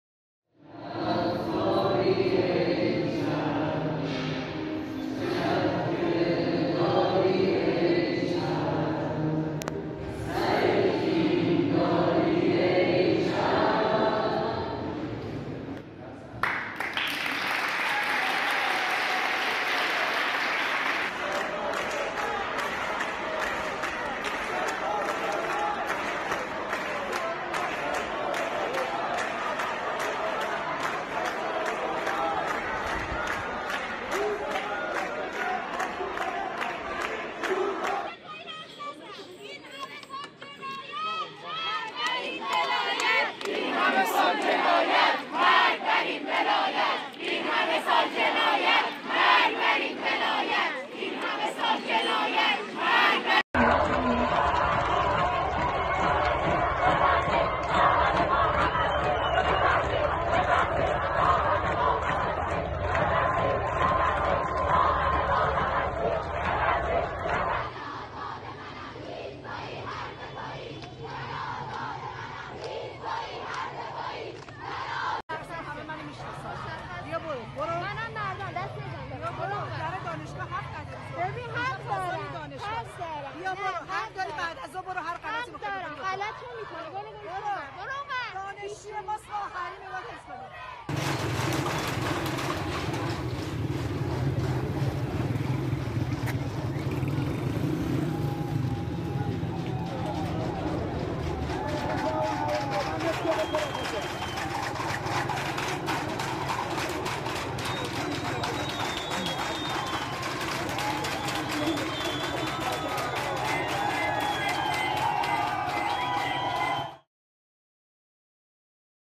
"I am a free woman": Students in Iran protest 40 days after Mahsa Amini's death
University students across Iran staged protests marking the 40-day mark of Mahsa Amini's death on Wednesday.

The 22-year-old Kurdish-Iranian woman died on Sept. 16 after falling into a coma while in the custody of Iran’s so-called morality police, where she was held for apparently not wearing her hijab properly according to the rigidly enforced Islamic Penal Code.

Students at Khajeh Nasir Toosi University of Technology in Tehran held a vigil for Amini, where they sang and lit candles. As well, students at Rafsanjani University Complex clapped and chanted in protest.

Plus, students at Ferdowsi University of Mashhad's Faculty of Engineering chanted: "Death to this velayat, all these years of crime." Velayat-e faqih is said to be a Shia Islamist system of governance that justifies the rule of the clergy over the state.

At Alzahra University, students chanted "I am a free woman, you are a whore," to mean "men are immoral, not women," as part of an ongoing protest.